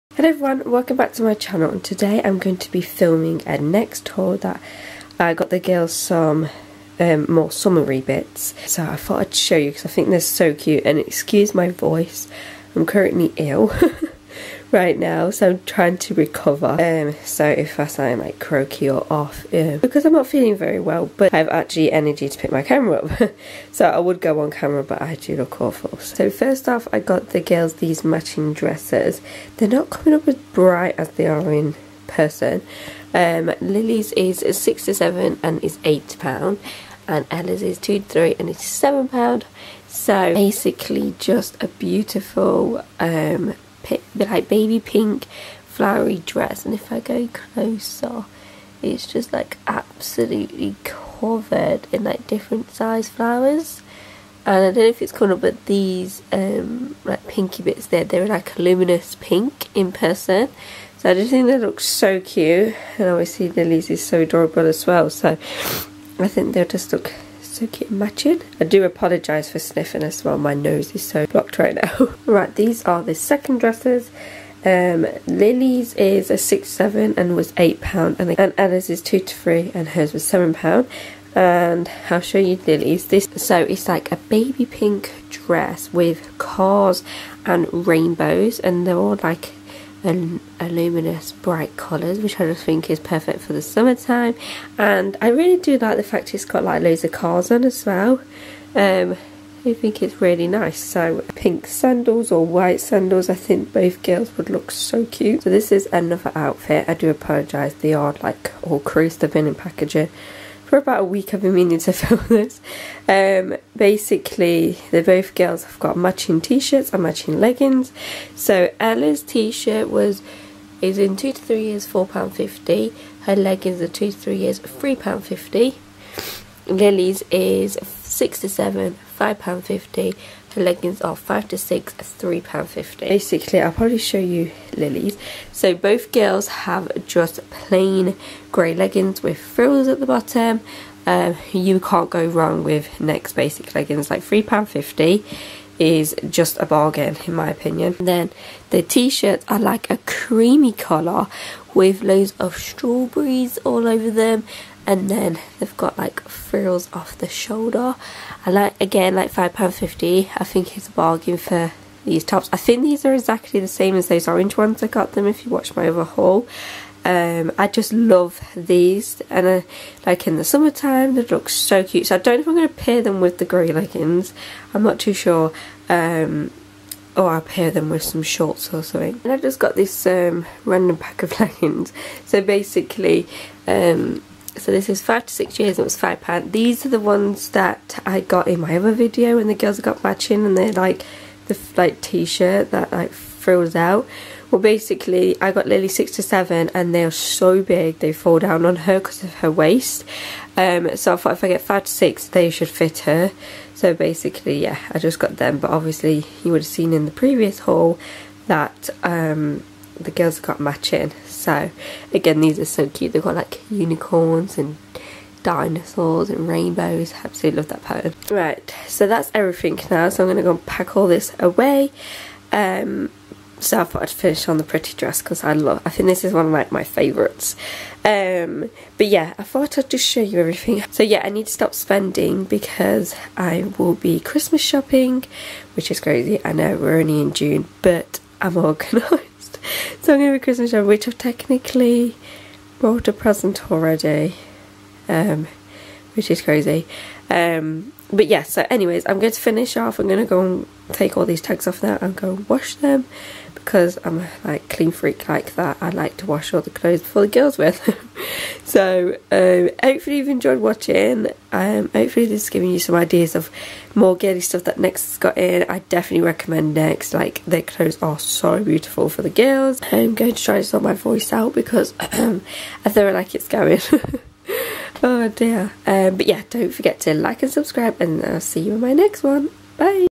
Next Summer Haul
Hello everyone this was filmed back in June and I forgot I didn't edit it, sorry about that. I thought I would still show you all since they are such lovely clothes.
Thanks Imogen X